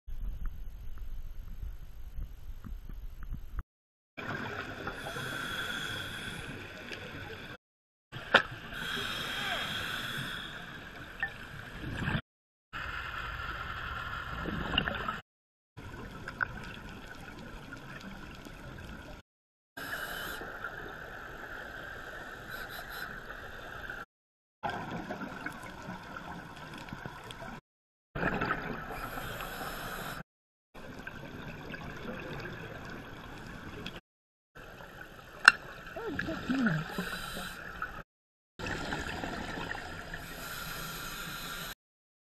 Bagging Fish
Lots of lionfish shooting and other cool creatures. Nice upside down and backwards spear shot.